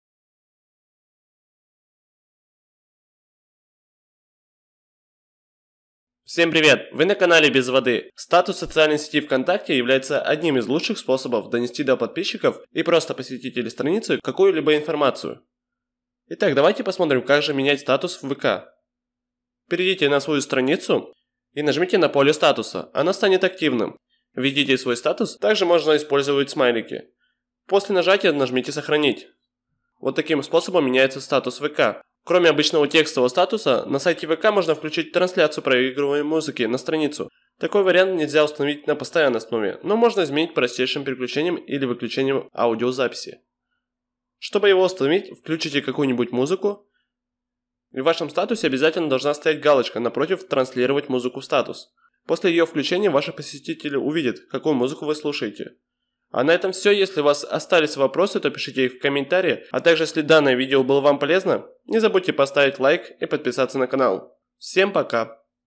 Как в статус в ВК поставить музыку
Всем привет друзья! В этом ролике я расскажу Вам Как в статус в ВК поставить музыку